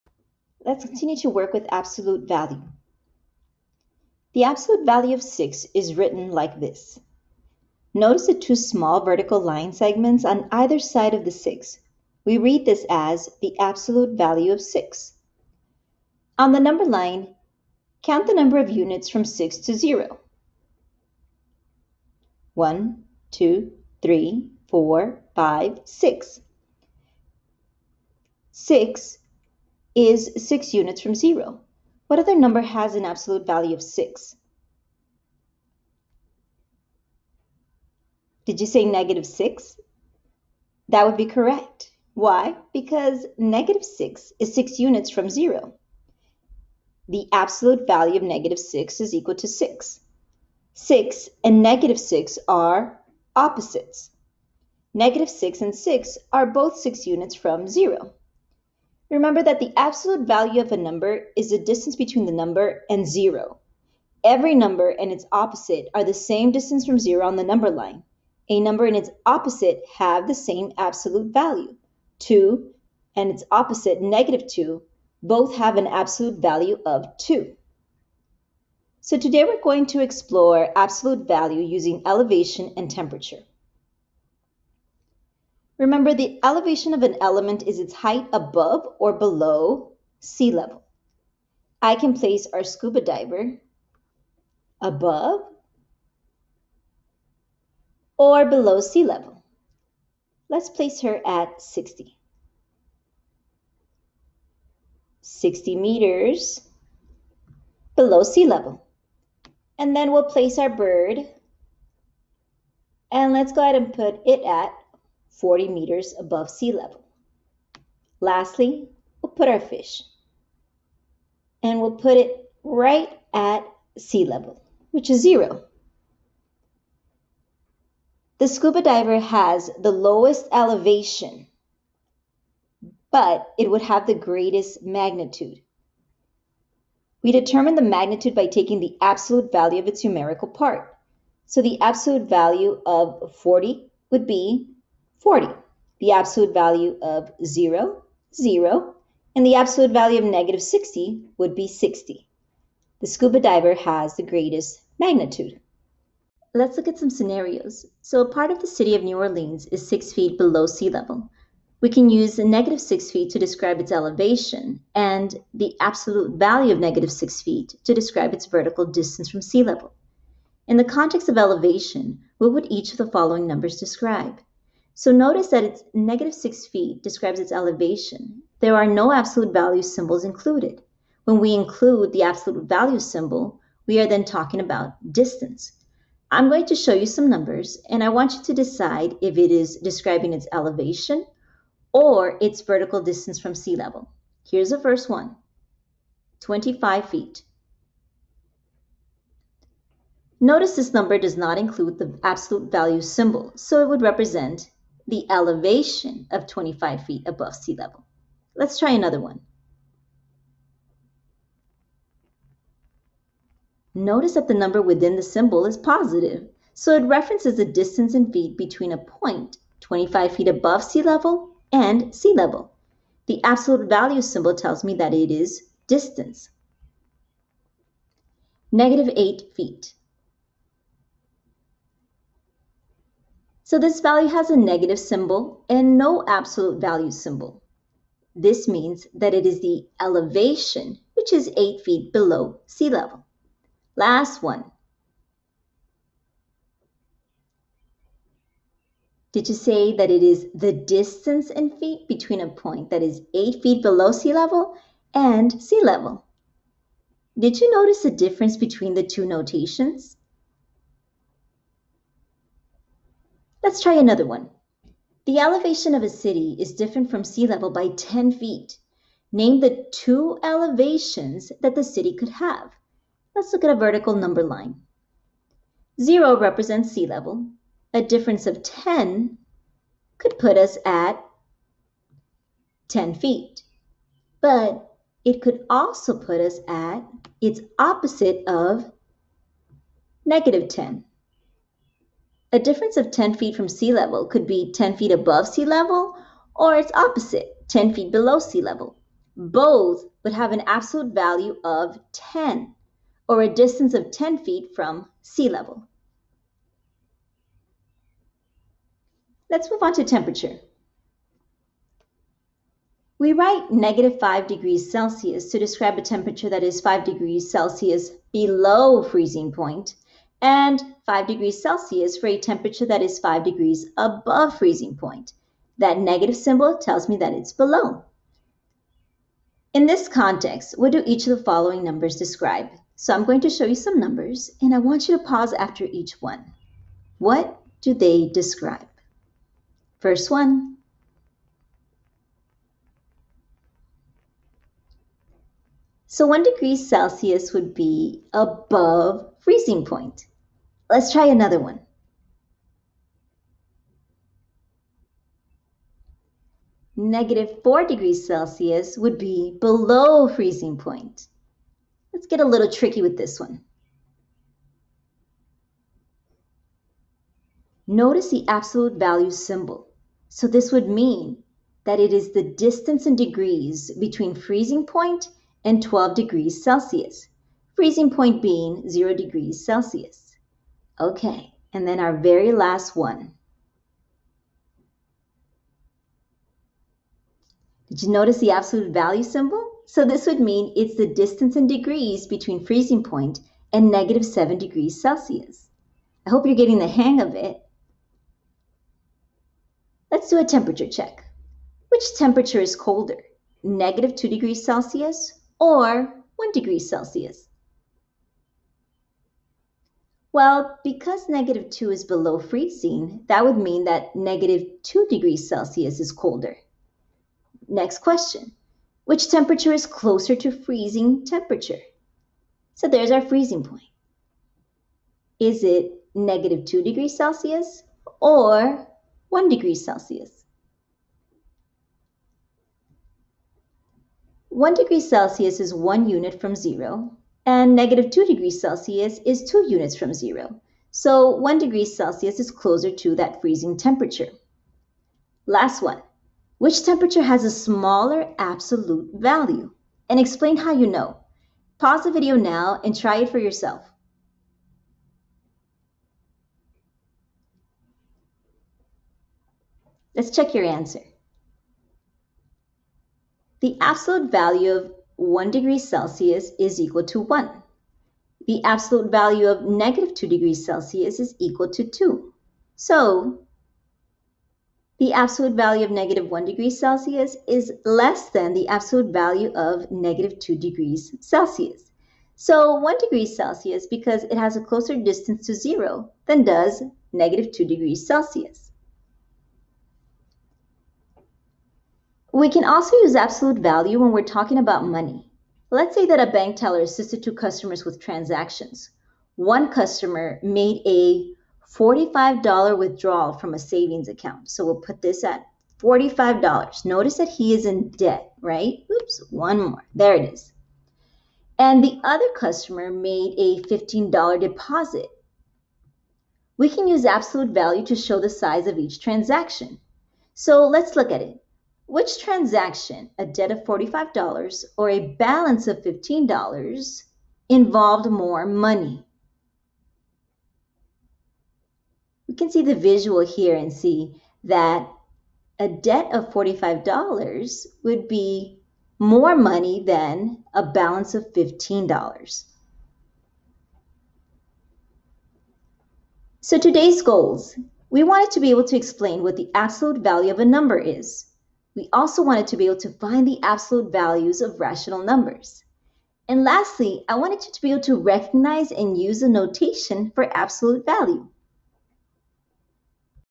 Lesson 6
Students now formalize the concept of a number’s magnitude with the term absolute value. They learn that the absolute value of a number is its distance from zero, which means that opposite numbers have the same absolute value. Students reason abstractly about the familiar contexts of temperature and elevation using the concept and notation of absolute value (MP2). OpenUp Resources